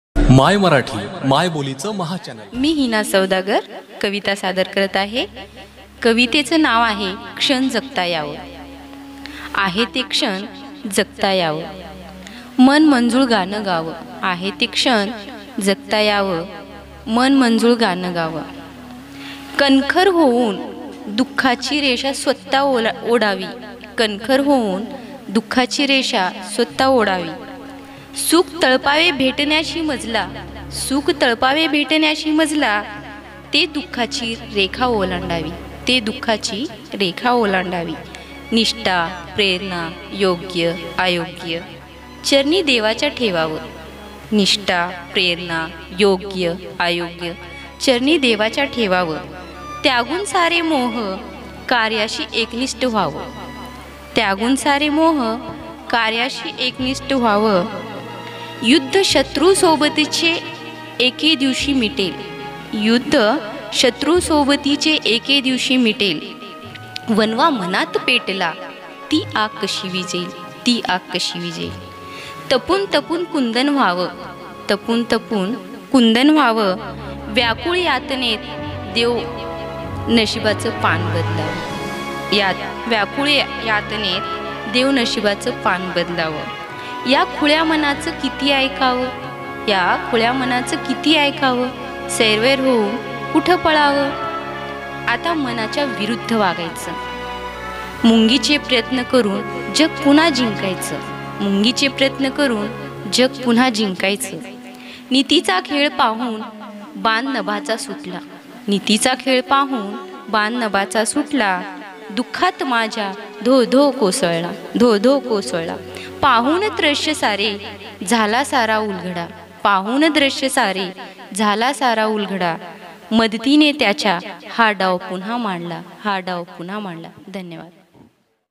kavita,हिना सौदागर,heena saudagar,मराठी कविता, बाप कविता,bap kavita,marathi poem on love,gazal,गझल,
हिना सौदागर  ..  अमराठी कुटुंबात जन्म घेऊनही मराठीत दमदार लेखन करणारी युवा कवयित्री. लातूरच्या उदगीर येथे शिक्षिका म्हणून कार्यरत असणारी हिना मराठीसह हिंदी,उर्दूतही छान लिहितेय.
मनाचा प्रवास रेखाटणारी हिनाची ही सुंदर कविता खास आपल्यासाठी.

'माय मराठी '.
मायबोलीचं महाचॅनल. !!

mai Marathi...
the biggest channel of the mothertoung.
-rajesaheb kadam